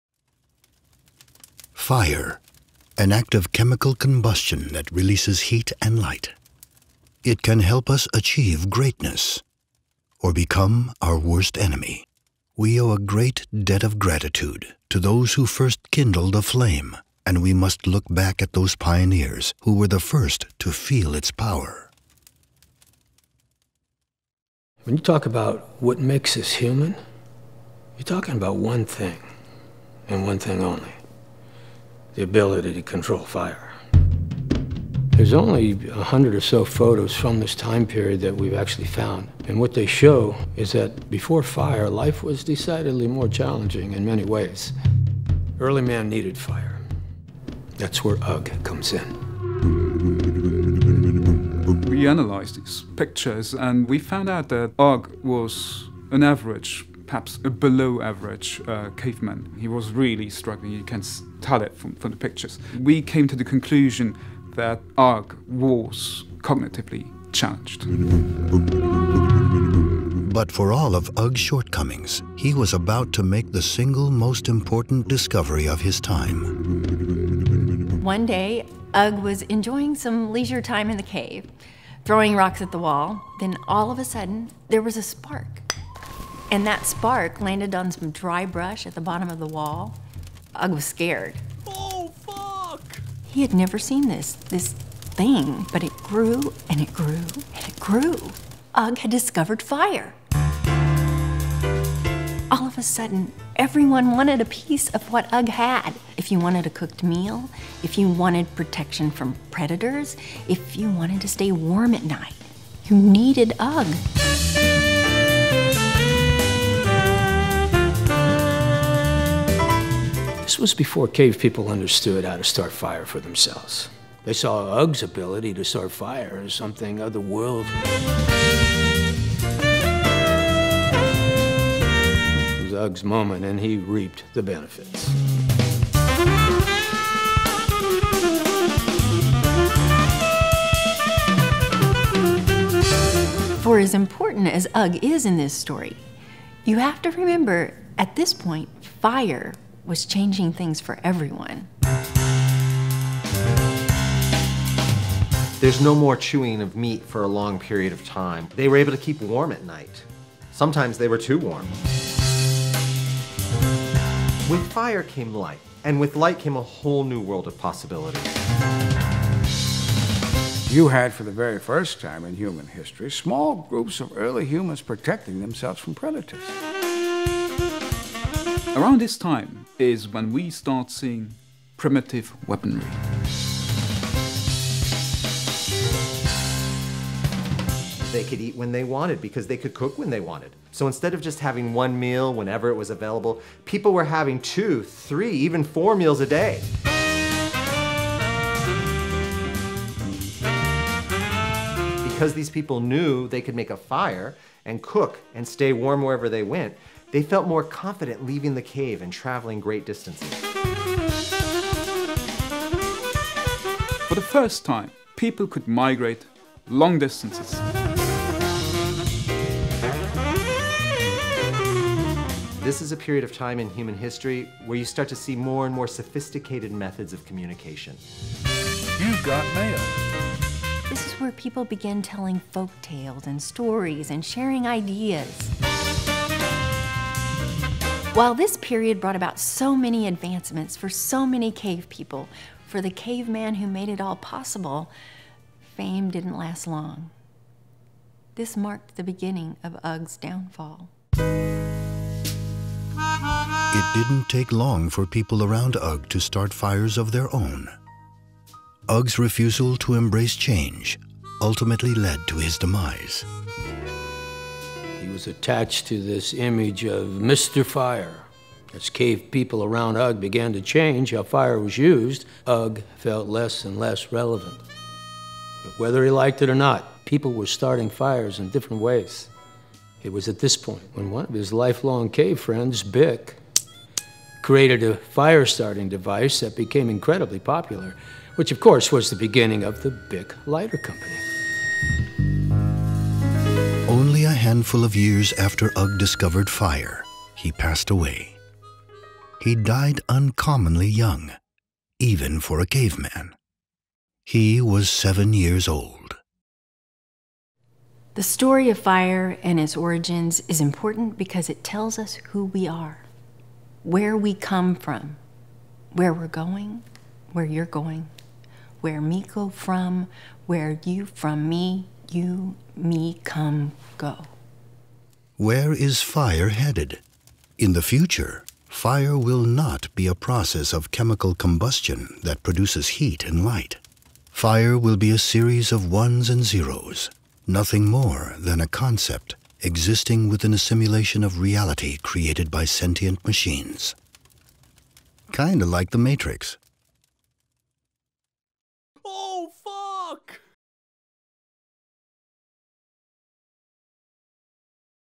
Fire: a Ken Burns Film
Meet the caveman who discovered fire in this Ken Burns documentary.

- Credits -
written, produced & directed by Andrew Heder & Ryan Kelly
shot & edited by Ryan Kelly
graphics & photoshop by Amy Bury

- Cast -
Voiceover by Yabo Obien
Thomas Abney - Don Baldaramos
Susan McDavid - Tamela Ivens
Sebastian Olivo - David Shorter
John Zimmerman - Juan Aguilar
Henry Eldridge - Ted Ferguson
Ugg - Andrew Heder  @andrewheder
Bic - Ryan Kelly  @RyanKelly______
Blonde Cavewoman - Amy Bury   @AmyBury1
Brunette Cavewoman - Melissa McIntosh   @McMelster

- Mommy Comedy -
Andrew Heder   @andrewheder
Ryan Kelly   @RyanKelly______

special thanks to Mofilm Studios for the studio space